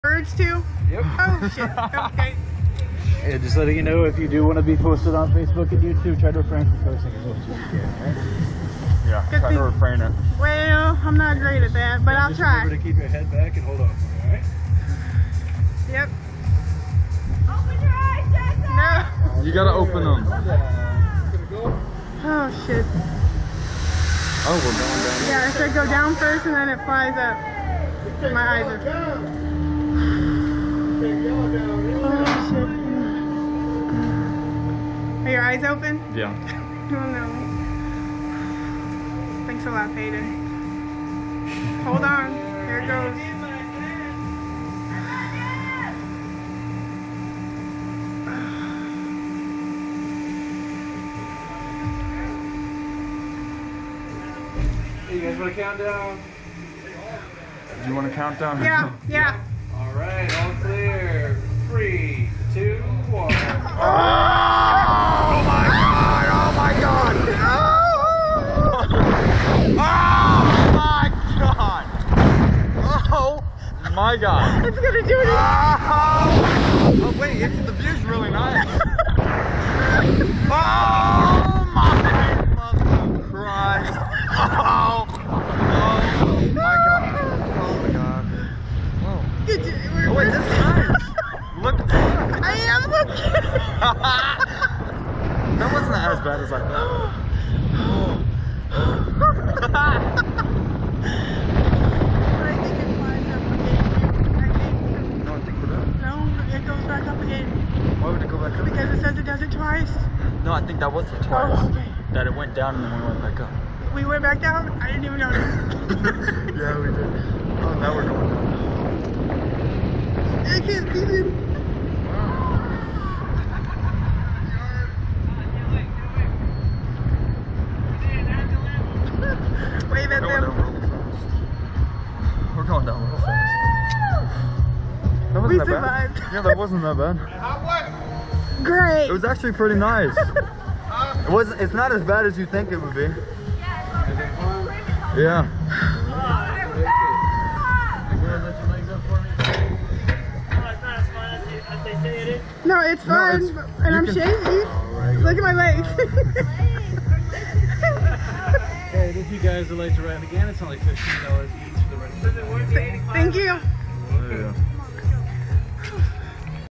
Jessie & Paydon
400' tall attraction that launches riders at 100 m.p.h.